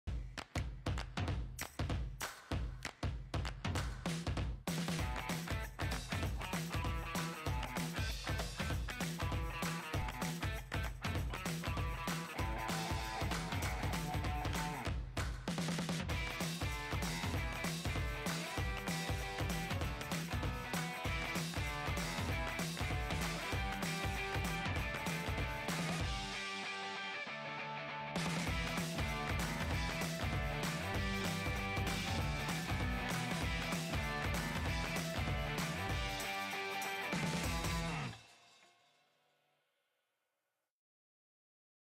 Indoor Touch Challenge 7 Brazilian Toe Taps
Brazilian toe taps

Steps:
1. Tap the ball with sole of foot
2. Tap the ball with other sole of foot
3. Stop the ball whilst putting leg behind standing foot (rabona touch)
4. Repeat
Hint: make sure to move side to side, you should move more than the ball

How many touches can you do in 30 seconds? Let us know! ⚽️👍 Share to friends and family, test yourselves against others!